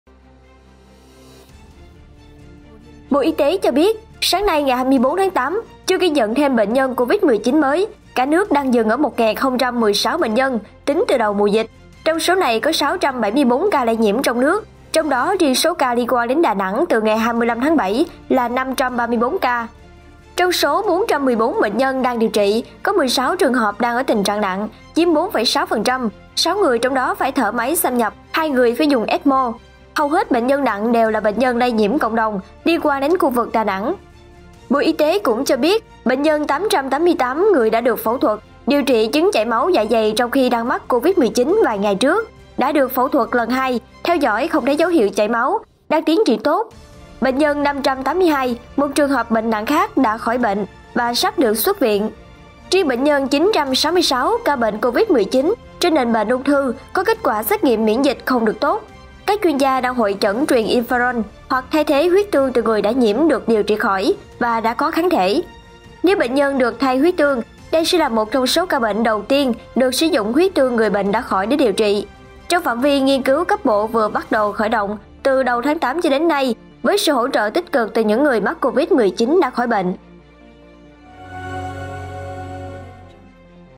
Chuẩn bị dùng huyết tương điều trị bệnh nhân đầu tiên ở Việt Nam
Sáng nay 24-8, Bộ Y tế thông báo không có thêm ca COVID-19, đây là buổi sáng thứ 4 không có thêm ca mắc mới, dù trong thông báo dịch chiều hàng ngày vẫn có bệnh nhân nhưng số ca mắc mới ghi nhận đã giảm rất mạnh.